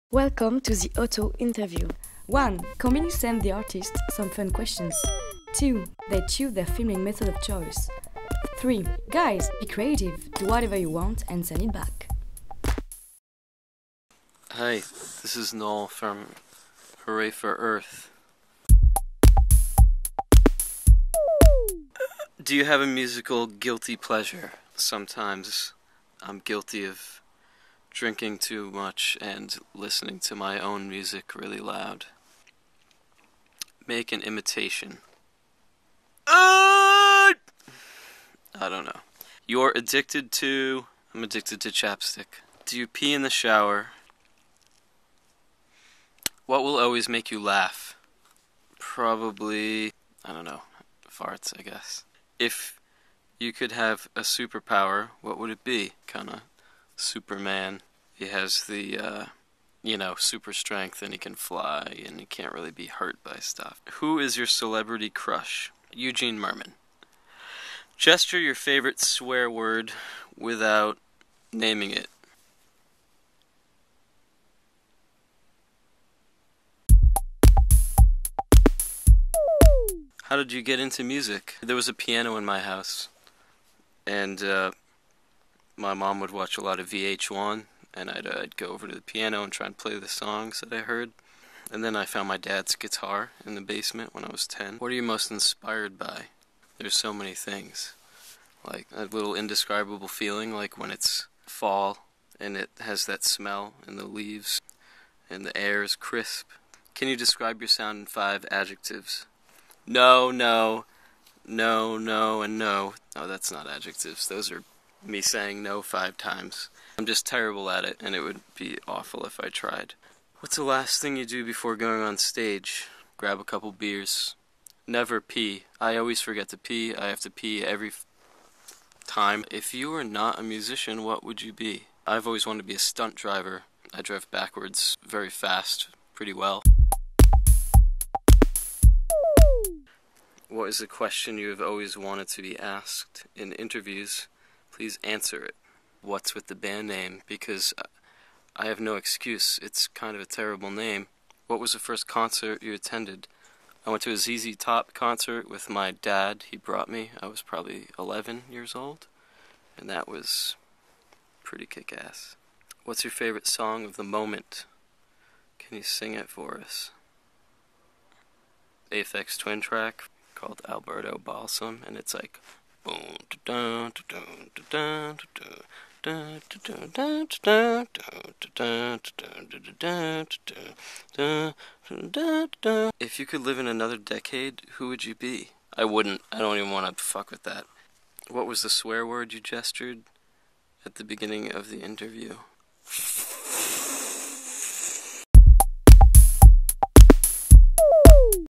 The Auto-Interview : Hooray for Earth
Noel Heroux aka Hooray For Earth made us an auto-interview from (we think) his bed in NYC. He added some gloomy effects, but he's sure not a gloomy guy!